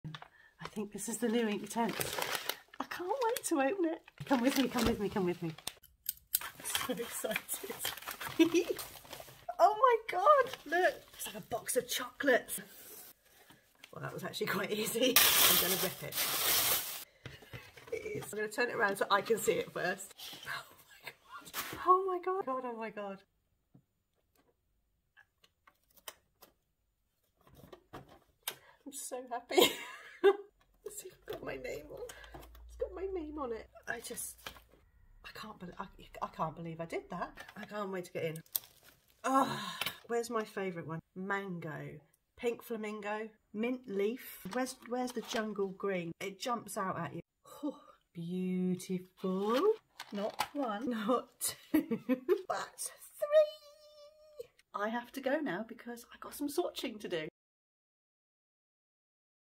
UNBOXING Derwent 100 Inktense Pencils - My artwork looks AMAZING!
Enjoy my genuine reaction as I unwrap my box of Derwent 100 Inktense Pencils and see my artwork on the tin.
Absolutely the best feeling ever!
Thankyou Derwent for this incredible opportunity.

You can find more of my art on Instagram, TikTok and Facebook @helencarterartist.